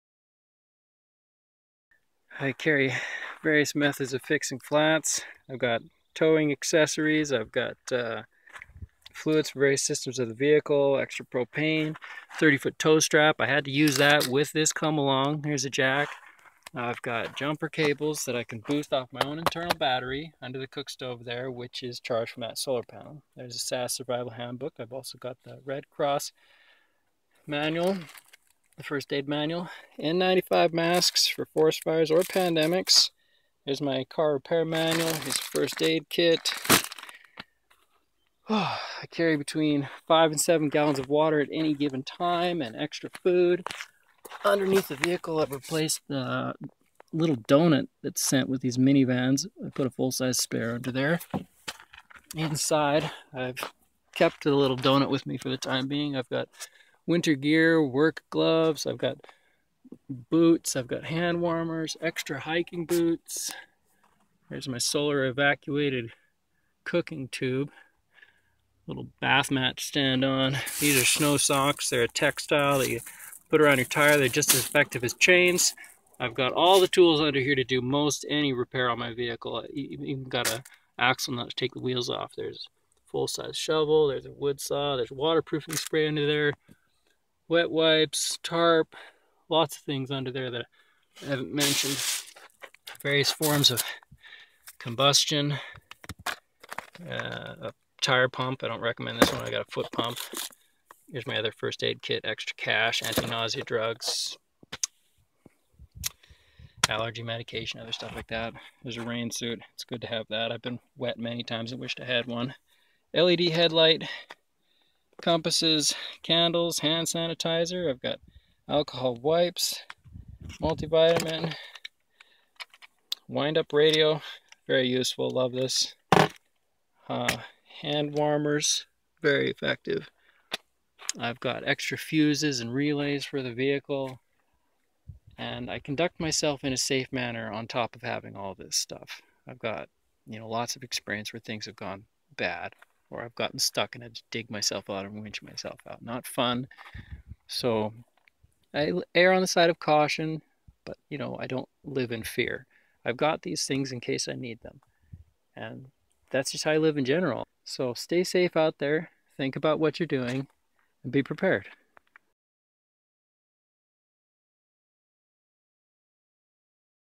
Prepare for anything on long road trips
How I stay ahead of disasters and inconveniences living in my van and on long road trips.  The items mentioned have kept me from getting stranded and safe.